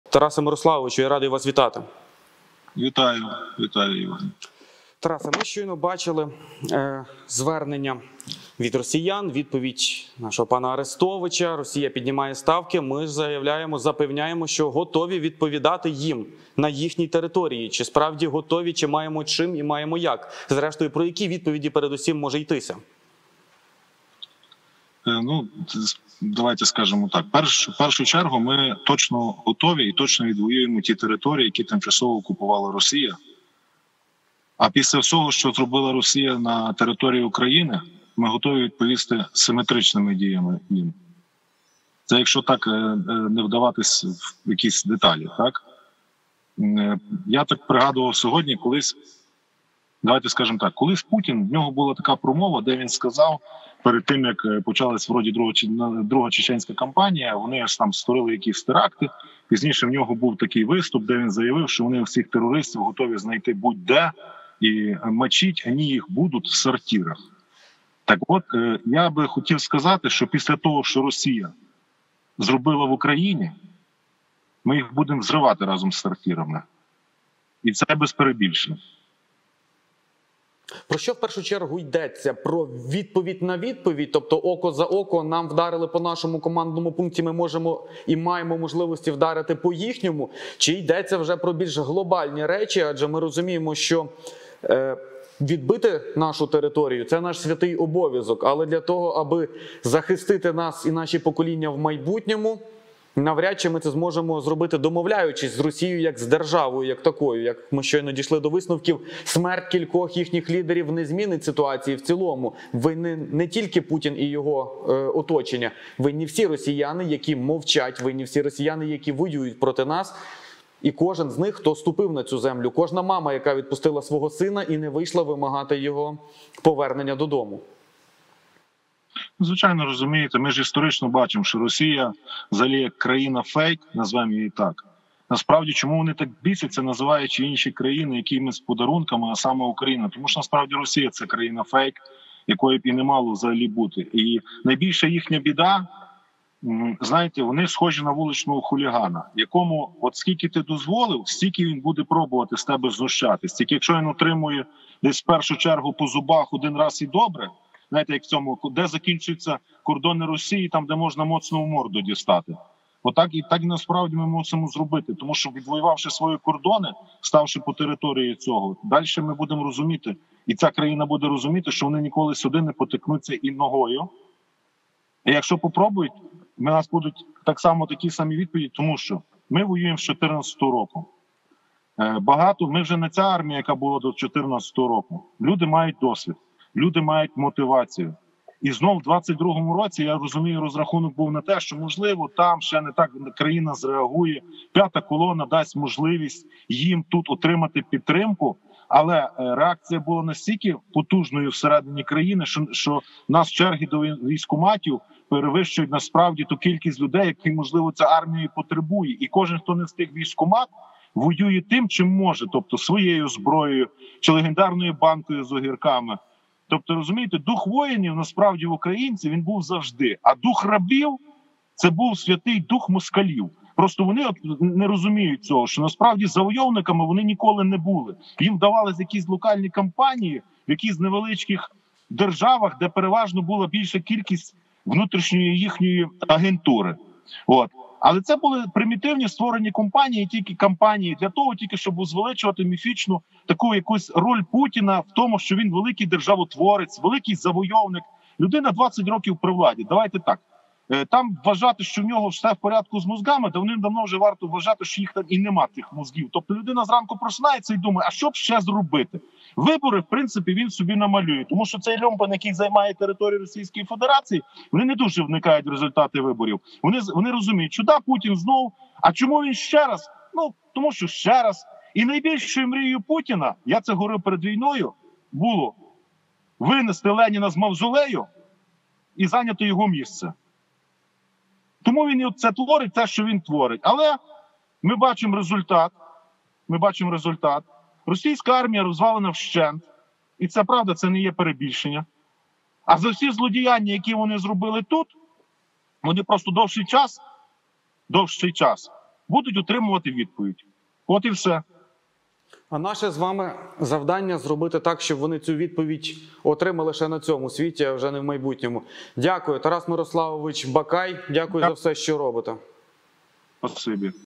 "МИ ЇХ БУДЕМО ВЗРИВАТИ РАЗОМ ІЗ СОРТИРАМИ" | Ukraine.Media
"МИ ЇХ БУДЕМО ВЗРИВАТИ РАЗОМ ІЗ СОРТИРАМИ" - ТАРАС БАКАЙ, військовослужбовець ЗСУ
Тарас Бакай з 2014 року воював у зоні АТО в лавах добровольчого батальйону “Свята Марія”. У 2017 році повернувся до Львова і обійняв посаду Заступника начальника Головного управління Національної поліції у Львівській області.

Ukraine.Media
Lviv.Media